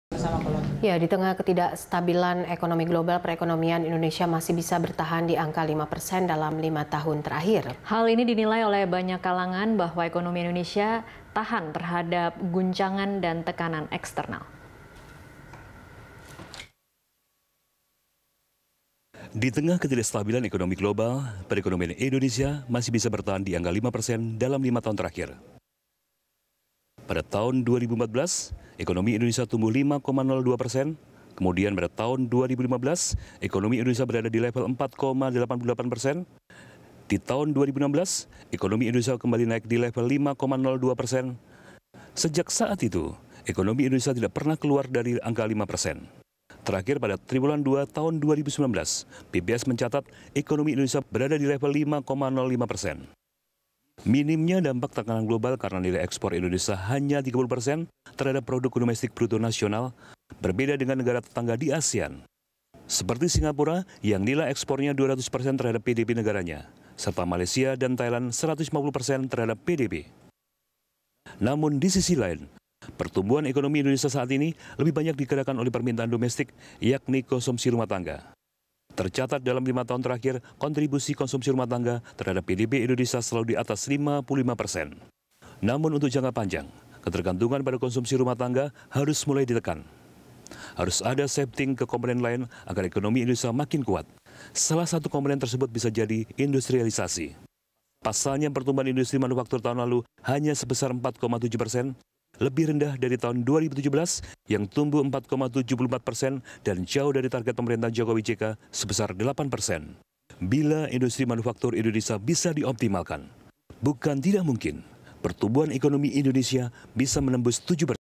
Perekonomian Indonesia Tahan Tekanan Eksternal
Di tengah ketidakstabilan ekonomi global, perekonomian Indonesia bisa bertahan di angka 5% dalam lima tahun terakhir.

Facebook.com/BeritaSatu
Youtube.com/BeritaSatu